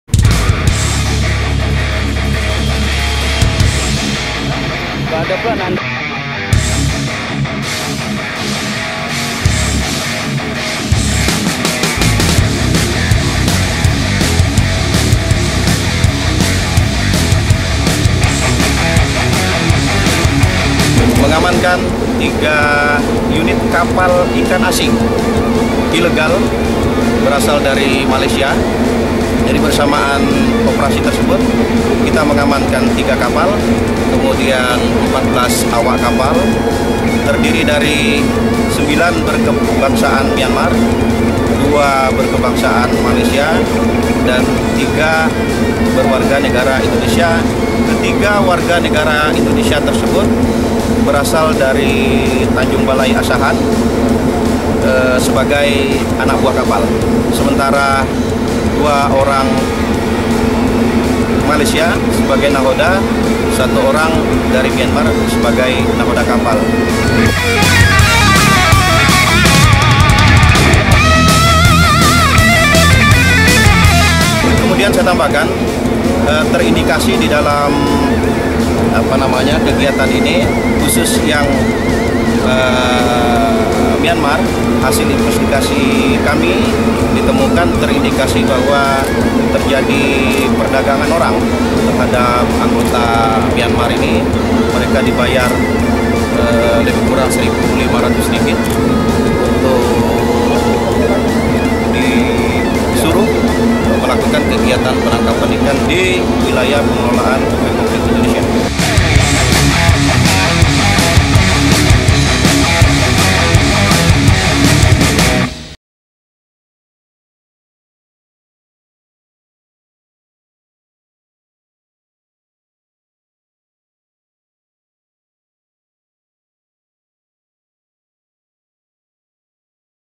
Curi Ikan di Indonesia, 3 Kapal Malaysia Ditangkap
Laporan Reporter Tribunnews Video, Hadi Maulana

BATAM.TRIBUNNEWS.COM, BATAM – Pengawasan Sumber Daya Kelautan dan Perikanan
 (PSDKP) Batam, Senin (7/3/2016) kembali mengekspos tangkapan kapal pengawas KKP yang berhasil menangkap tiga kapal ikan asal Malaysia yang mencuri ikan di perairan Indonesia.

Dimana tiga kapal ikan asal Malaysia itu diantaranya SLFA 4625, KHF 1917 dan PKFB 1512, dengan anak buah kapal (ABK) 14 orang yang terdiri dari sembilan kewarganegaraan Myanmar, dua kewargaa negaraan Malaysia dan tiga kewarganegaraan Indonesia.

“Untuk yang Indonesia merupakan warga Tanjung Balai Asahan Sumatera Utara (Sumut) yang merupakan TKI di Malaysia,” kata Kepala PSDKP Batam, Akhmadon, Senin (7/3/2016).

Akhmadon mengungkapkan ketiga kapal asal Negara jiran itu tertangkap basah sedang melakukan aktifitas ilegal fishing (pencurian ikan) di perairan teritorial Indonesia.

"Ketiga kapal ini kita tangkap di Selat Malaka yang merupakan perairan Republik Indonesia," katanya.

Bahkan, ketika ditangkap ketiga kapal itu tidak dilengkapi dengan dokumen yang syah dari Pemerintah Indonesia. Selain itu, alat penangkap ikan yang digunakan adalah Trawl atau Pukat Harimau, yang secara hukum dilarang pengoperasinanya di wilayah Indonesia.

“Dari ketiga kapal tersebut sedikitnya ada 100 Kg ikan campuran hasil tangkapan mereka di perairan Indonesia,” katanya.

Dan ke 14 ABK tersebut akan dijerat pasal 5 ayat 1 huruf a, pasal 92 juncto pasal 26 ayat 1, pasal 93 ayat 2 juncto pasal 27 ayat 2, pasal 85 juncto pasal 9 ayat 1, UU No. 45 Tahun 2009 tentang Perubahan atas Undang-undang Nomor 31 Tahun 2004 tentang Perikanan dengan ancaman hukuman 6 tahun penjara.